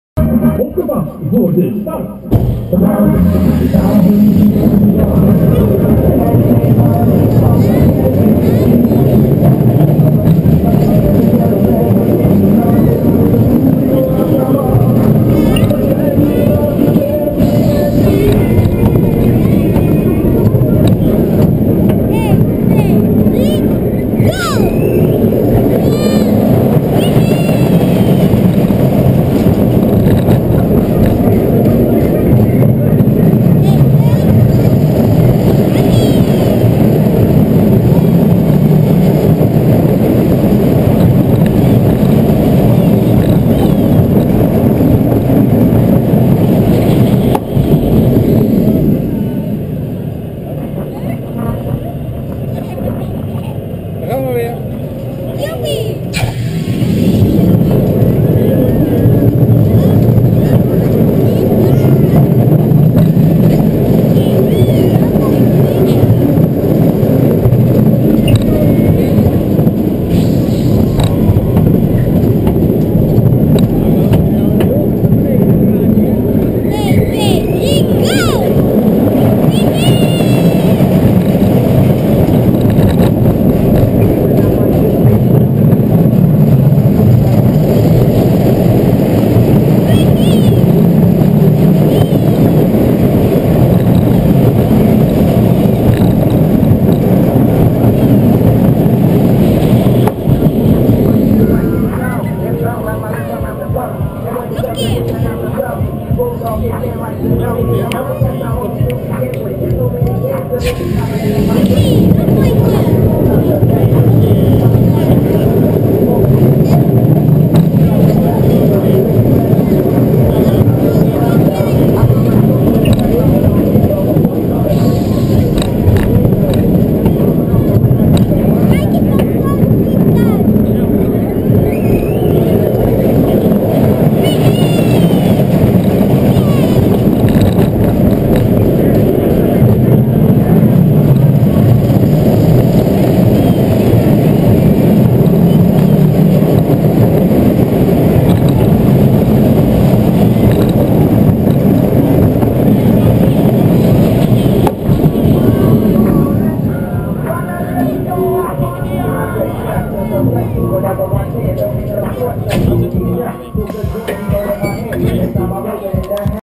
Zeeslang (Dippie Doe, NL)
Scherpe bochten en hoogteverschillen wisselen elkaar in snel tempo af. Ook voor de jonge durfhalzen is de achtbaan een hele belevenis.